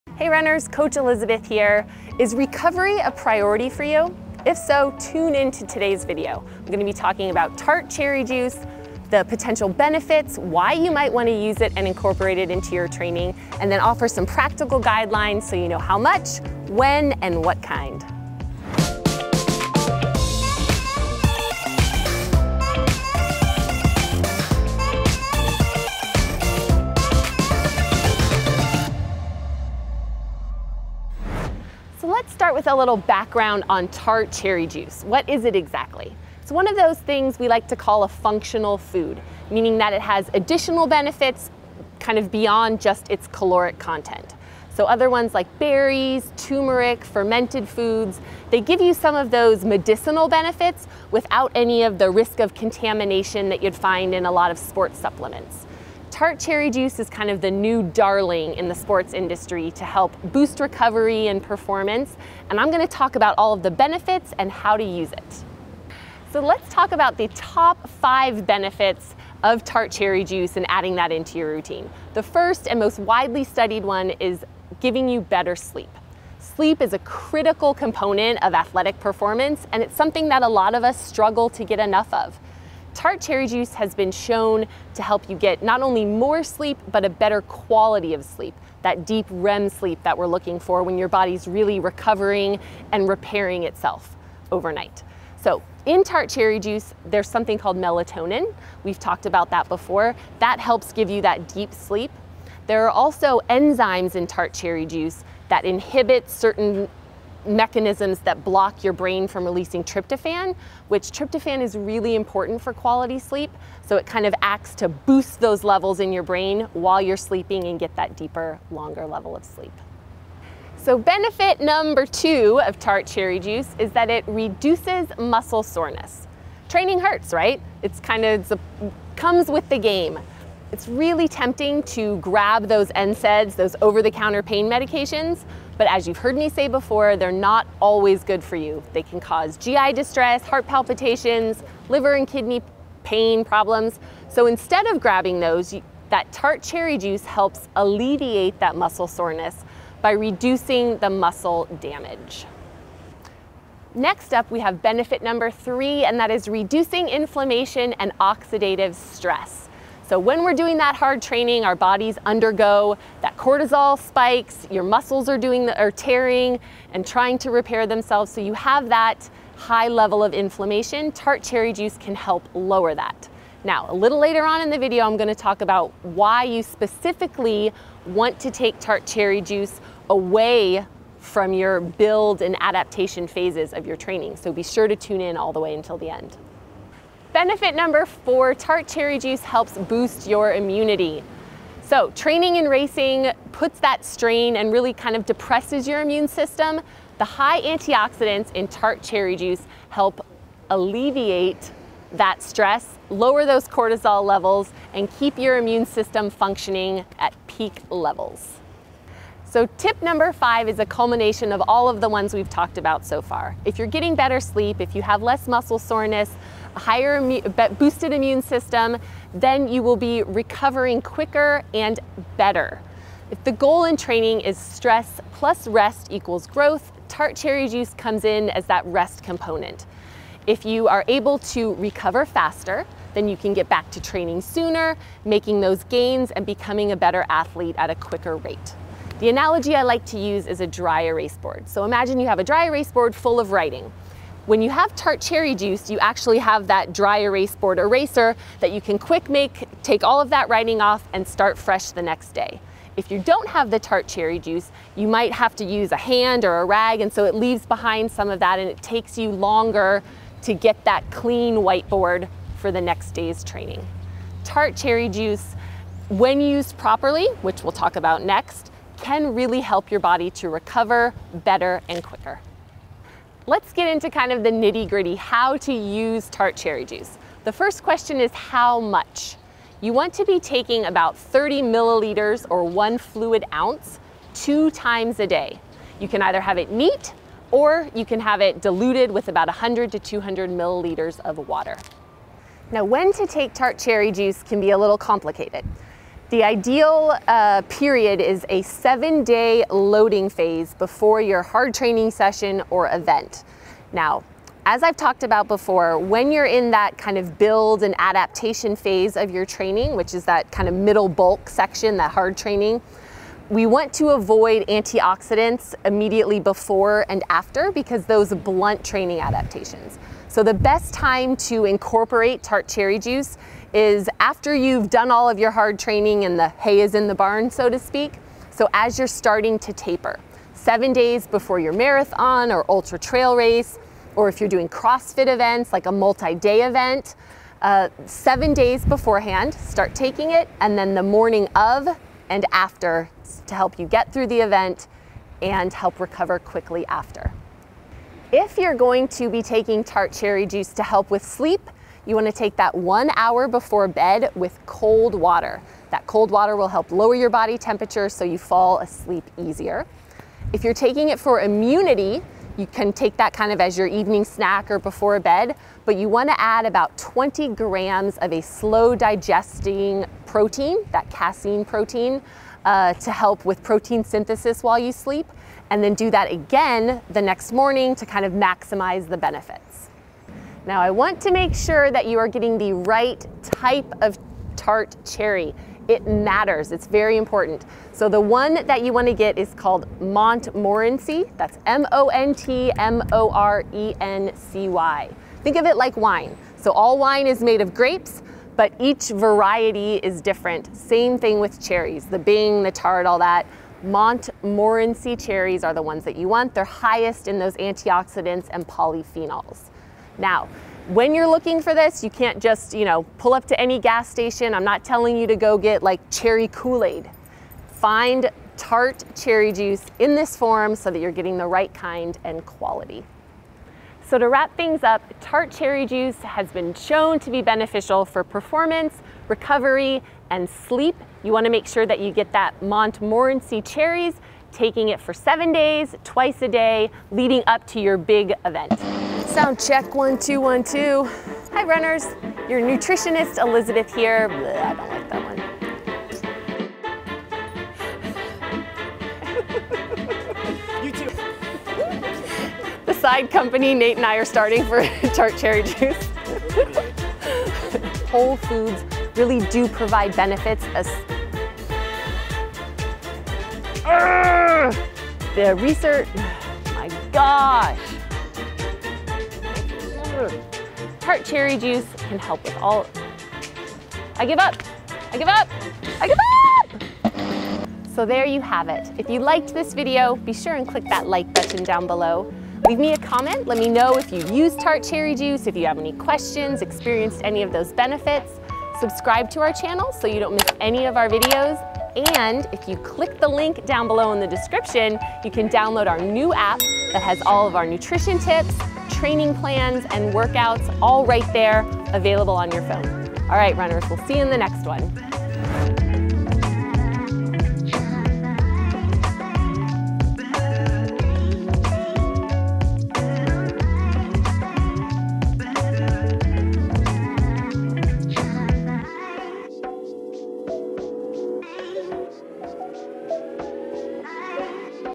Tart Cherry Juice | How It Affects Your Training And Performance
What's the deal with tart cherry juice and specifically, how it affects your training and performance? Coach Elizabeth dives into the benefits of this natural substance, the specifics of how and when to use it, as well as her favorite brands. Go, cherries!